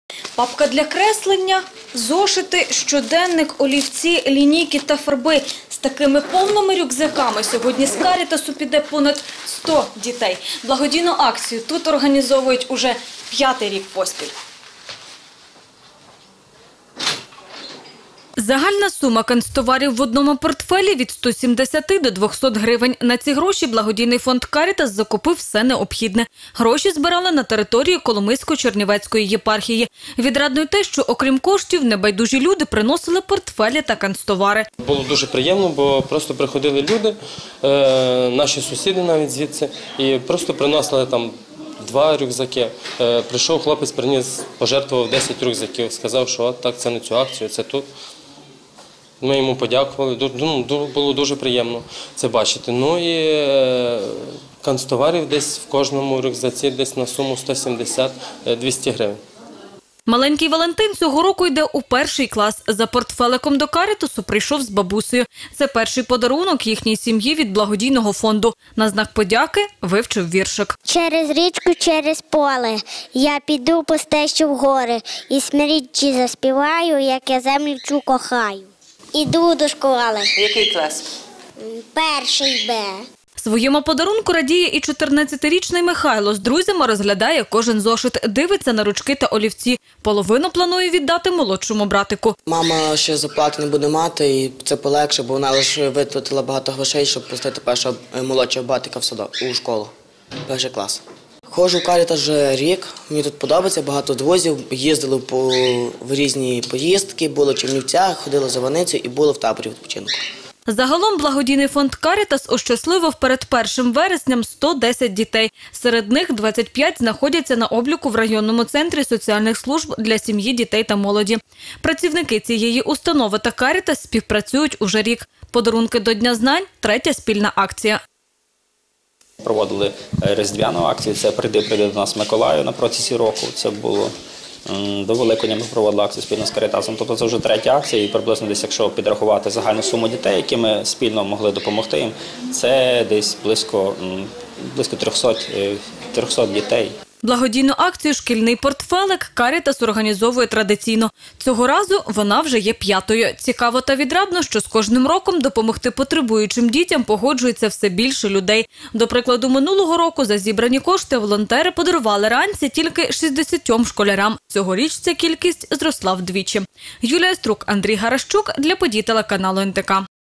Благодійна акція від "Карітас"
Сироти та малозабезпечені діти Коломийщини безкоштовно отримали портфелі з канцтоварами. Необхідні речі до школи учням вручив благодійний фонд «Карітас». Протягом місяця представники організації збирали кошти на потрібне шкільне приладдя в деканатах, церквах та на вулицях. Волонтерів підтримали небайдужі люди і загалом здали 20 тисяч гривень.